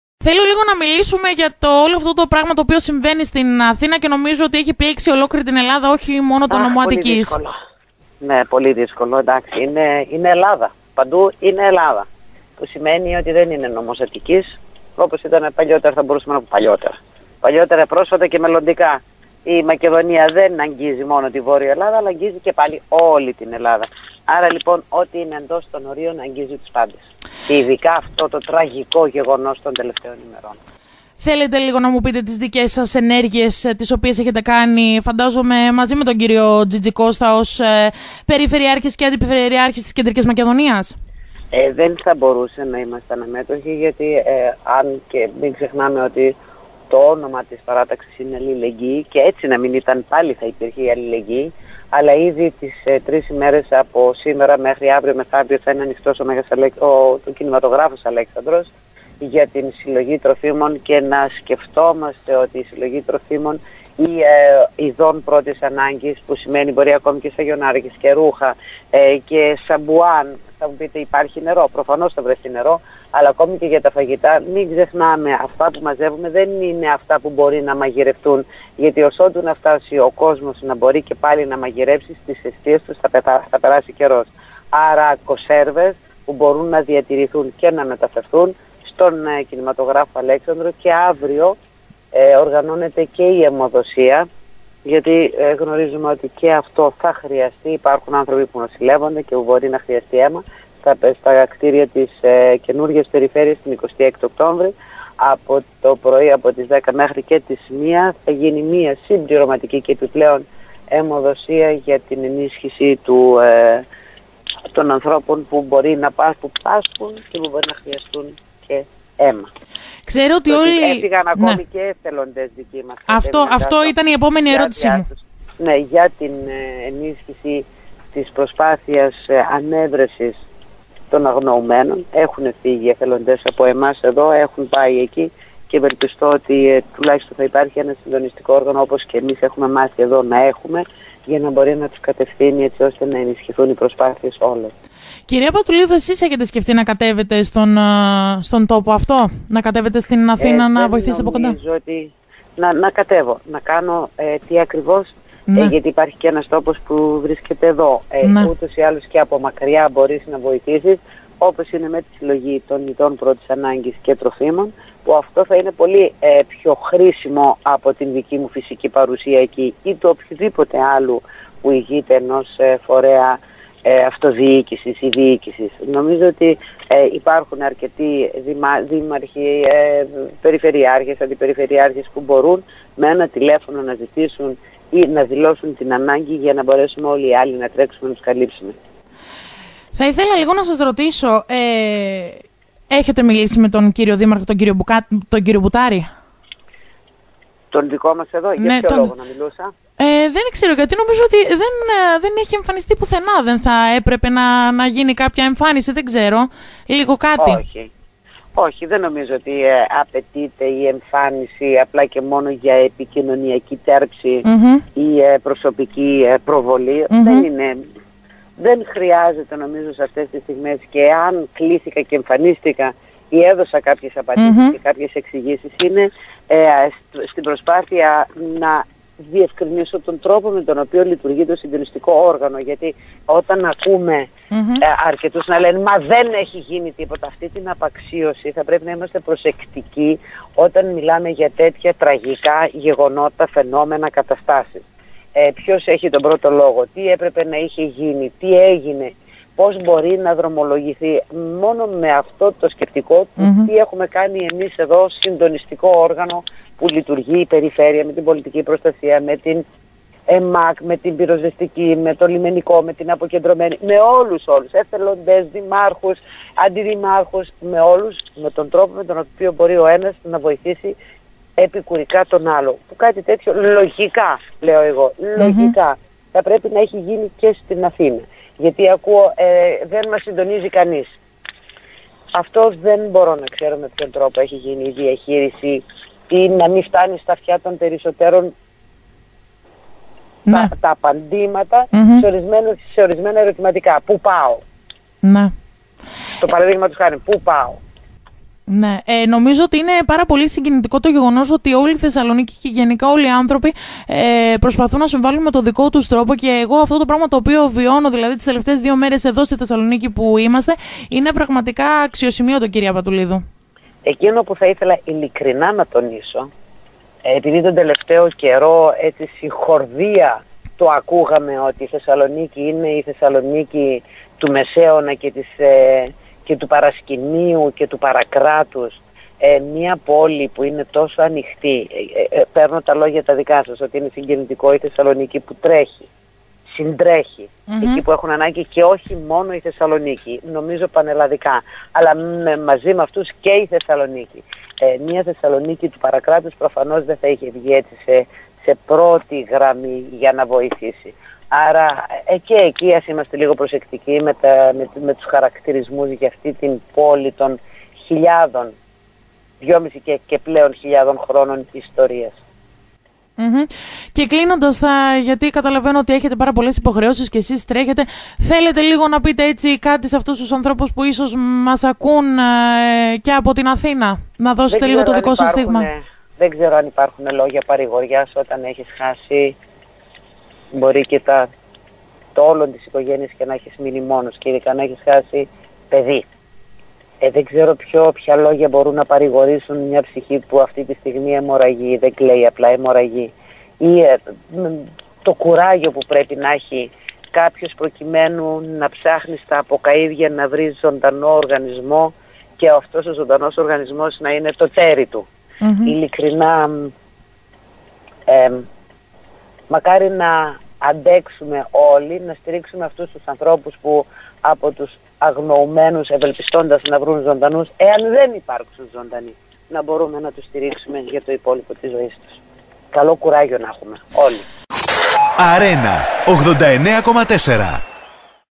Πατουλίδου: «Η Θεσσαλονίκη δεν είναι του παρασκηνίου,άλλα της πρώτης γραμμής»
Η Βούλα Πατουλίδου σε συνέντευξη της στο ARENA FM 89.4 δήλωσε την στήριξή της στους πληγέντες των πυρκαγιών της Δυτικής Αττικής αλλά και πως η Θεσσαλονίκη δεν είναι του Μεσαίωνα και του παρακράτους αλλά της πρώτης γραμμής. Επίσης απαντάει και για το γεγονός πως ο Δήμαρχος Θεσσαλονίκης Γ.Μπουτάρης δεν έχει κάνει ακόμα κάποια δήλωση. Συνέντευξη στην Ε. Ασπιώτη και τον Μ. Μουταφίδη